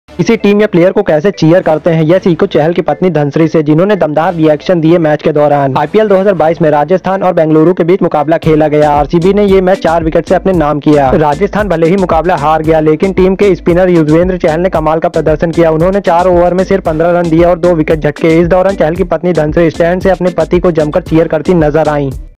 देखिये बीच मैदान चहल के विकेट पर धनश्री का डांस | IPL 2022
देखिये बीच मैदान चहल के विकेट पर धनश्री का डांस | IPL 2022 RR vs RCB

Ipl highlights today 2022
Dhanshree varma dance when chahal takes wicket in rr vs rcb match